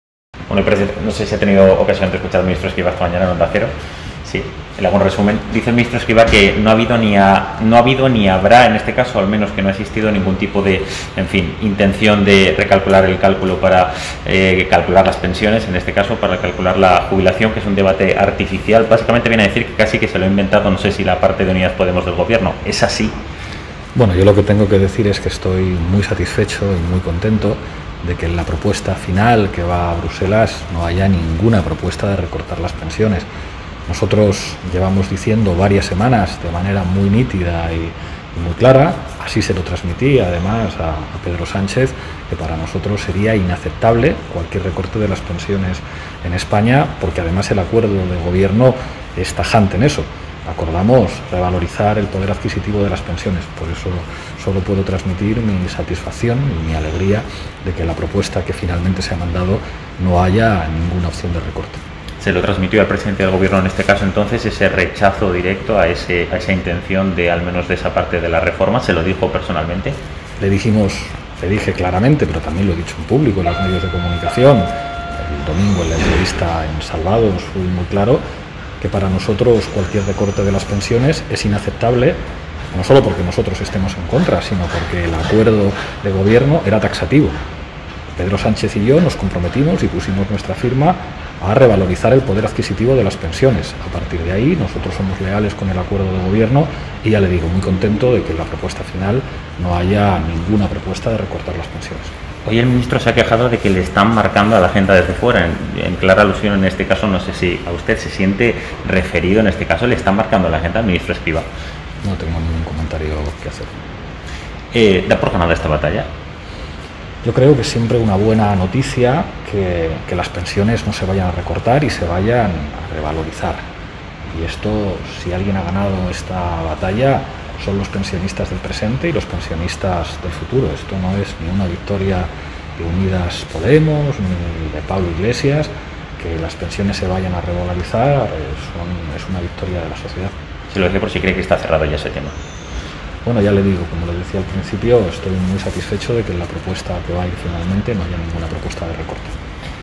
El debate sobre el recorte de las pensiones ¡CERRADO!🔒 | Pablo Iglesias
Pablo Iglesias pone fin al debate sobre la reforma de pensiones.